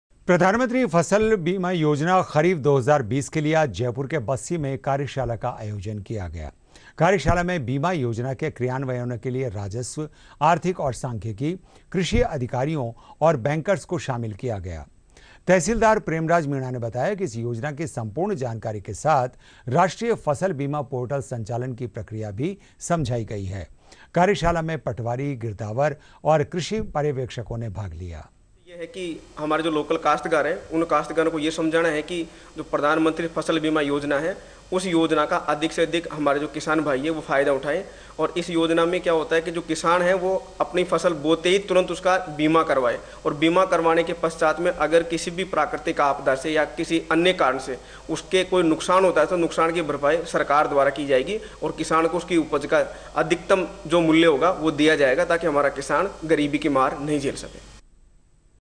प्रधानमंत्री फसल बीमा योजना खरीफ-2020 के लिए कार्यशाला आयोजित
प्रधानमंत्री फसल बीमा योजना खरीफ-2020 के लिए आज जयपुर के बस्सी में एक कार्यशाला का आयोजन किया गया। कार्यशाला में बीमा योजना के क्रियान्वयनों के लिए राजस्व, आर्थिक एवं सांख्यिकी, कृषि अधिकारियों और बैंकर्स को शामिल किया गया था। तहसीलदार प्रेमराज मीणा ने बताया कि इस योजना की संपूर्ण जानकारी के साथ राष्ट्रीय फसल बीमा पोर्टल संचालन की प्रक्रिया भी समझाई गई है। कार्यशाला में पटवारी, गिरदावर एवं कृषि पर्यवेक्षकों ने भाग लिया।